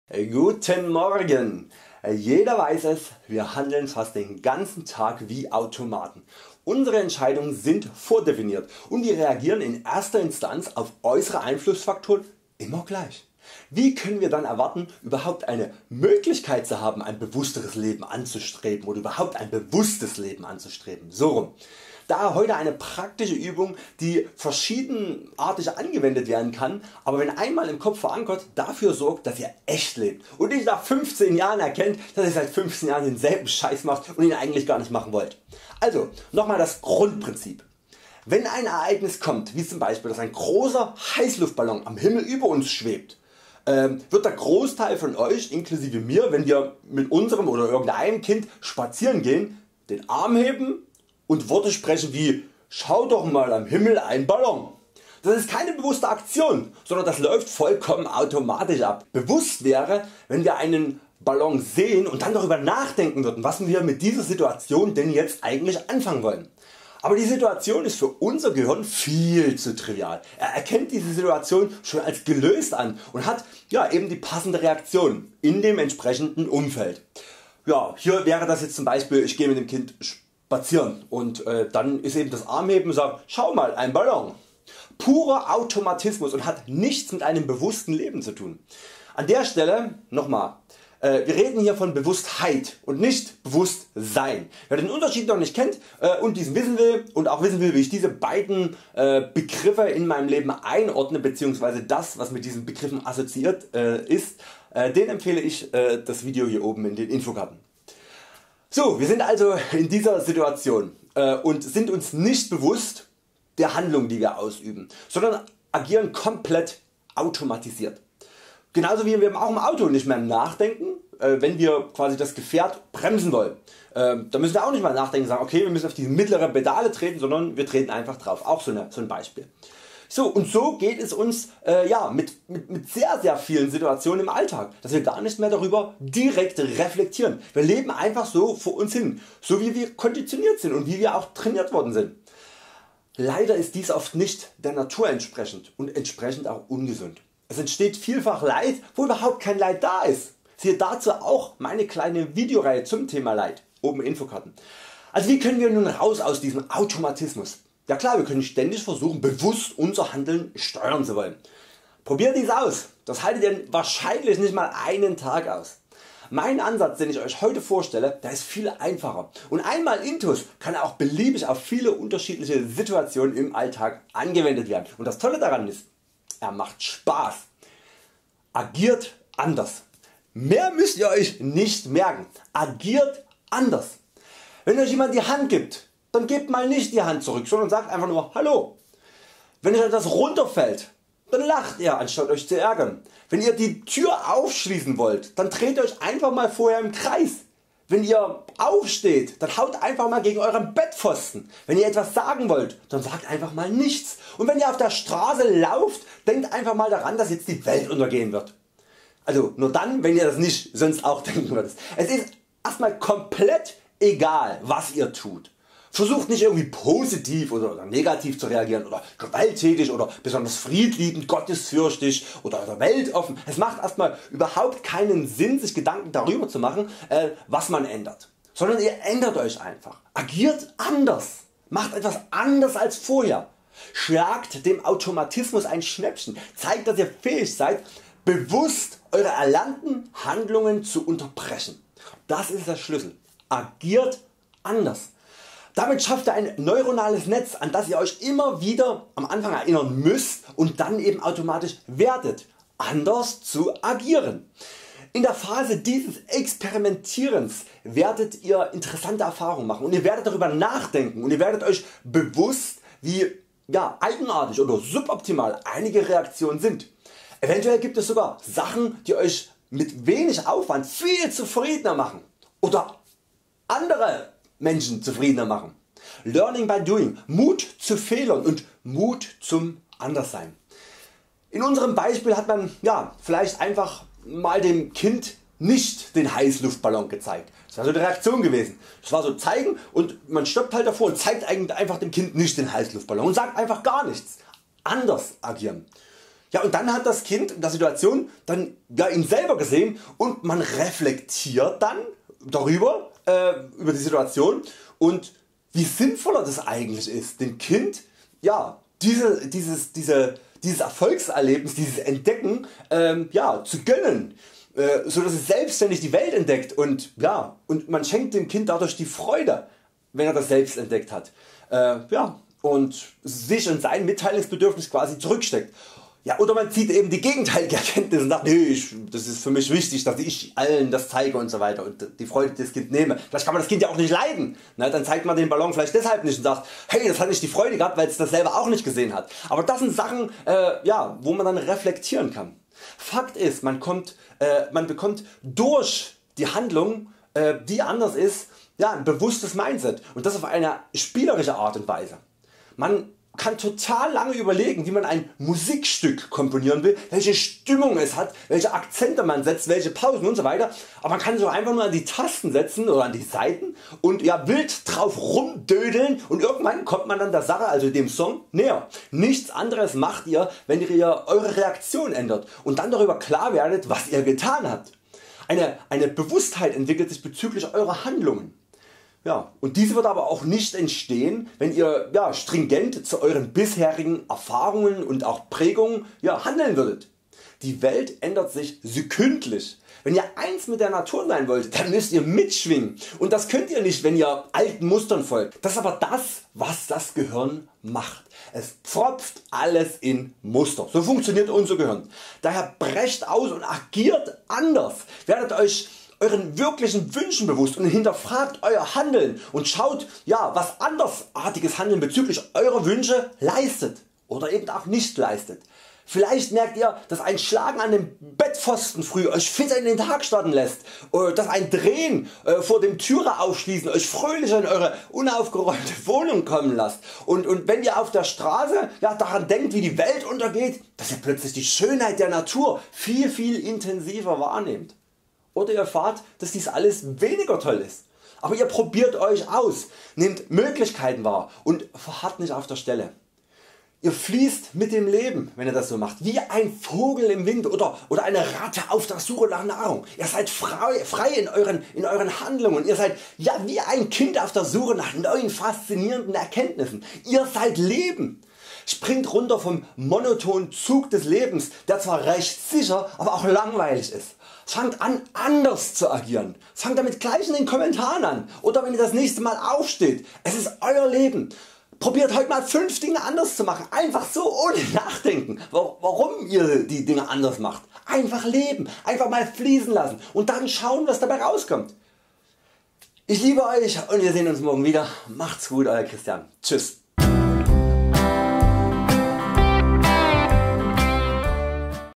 Sein Gehirn austricksen! Bewusst Leben, Psychologie für den Alltag! Leben ändern durch Bewusstheit!
Heute ein lustiger Trick wie man Bewusstheit im Leben steigert und sein Leben ändert! Trickst Euer Gehirn aus mit "Agiert anders!". Gestaltet Euren Alltag neu mit Freude und Reflexion! Viel Freude beim Video!

Wenn ihr mir einen Brief schreiben wollt, ist meine Adresse:

Christian Pörtzel
Friedrich Engels Strasse 55
04425 Taucha